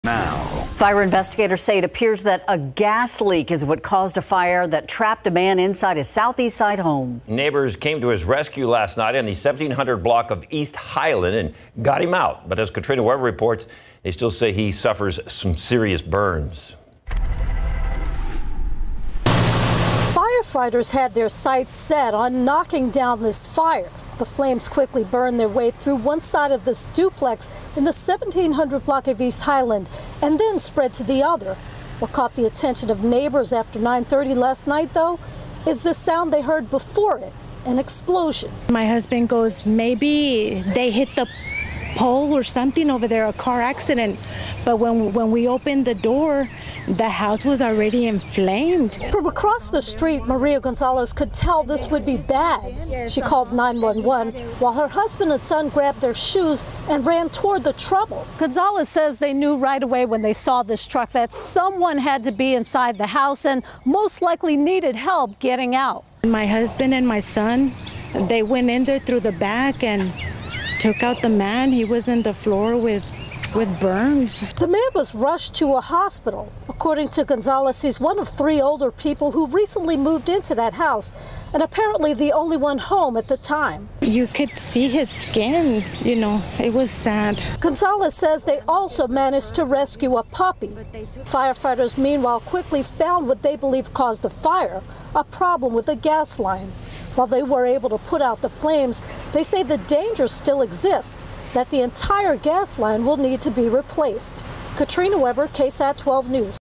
Neighbors describe rescue of man from fire, explosion in Southeast Side home
A Southeast Side man is in the hospital being treated for burns after being pulled from his burning home Sunday night.